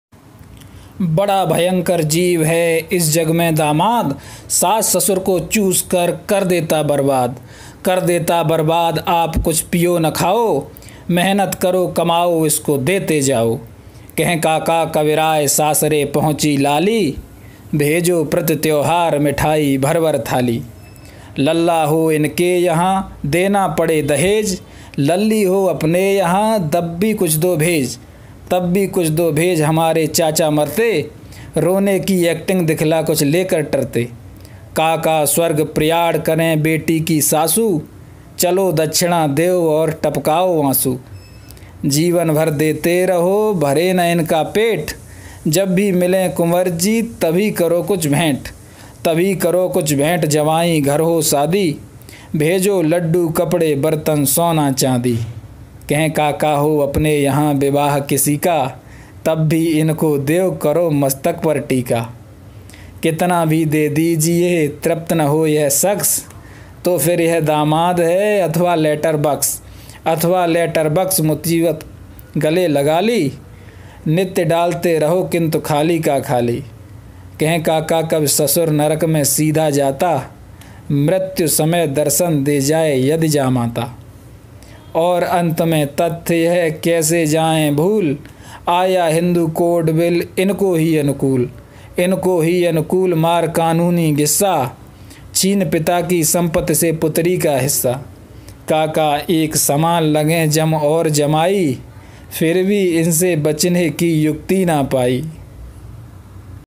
दामाद पर "काका हाथरसी" की हास्य कविता
इस वीडियो में काका हाथरसी की दामाद पर एक रोचक कविता को प्रस्तुत किया गया है।